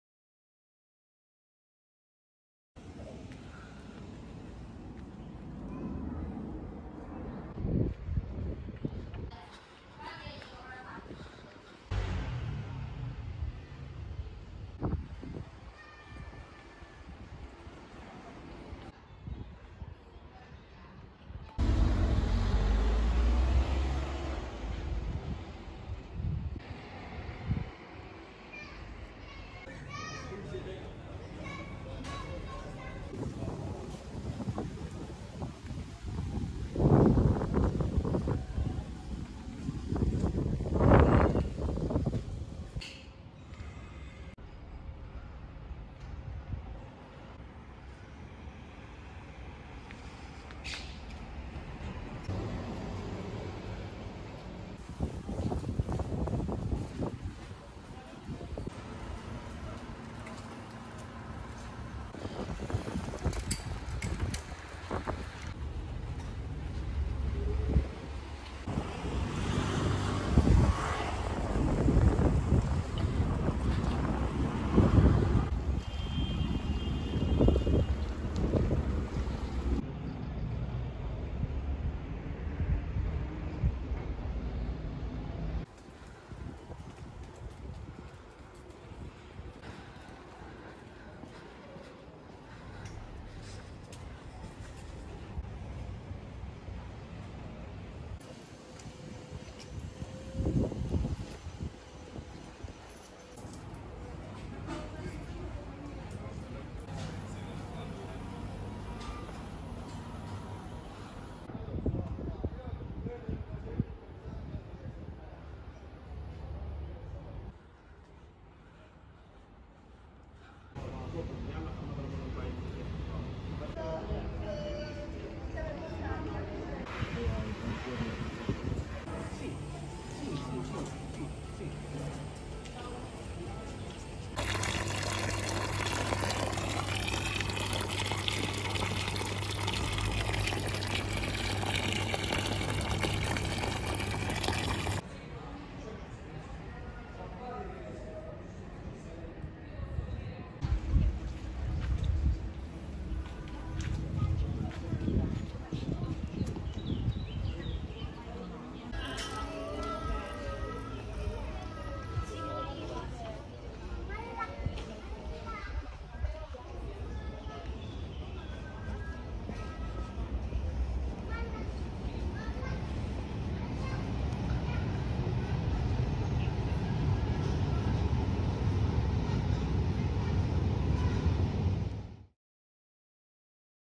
romangalgoesaround "Roman Life" #1 - Pigneto
I'm so excited to present you my new (and first) mini documentary project called "Roman Life" where I will be going around to show you the hidden gems of this city and you'll be able to soak in images and sounds of this incredible city. Every episode will be about a different neighborhood or a special place of Rome focussing on sounds, details and life scenes. In this first episode, discover with me the beauty of Pigneto. Walk through an amazing, colorful and lively neighborhood. :)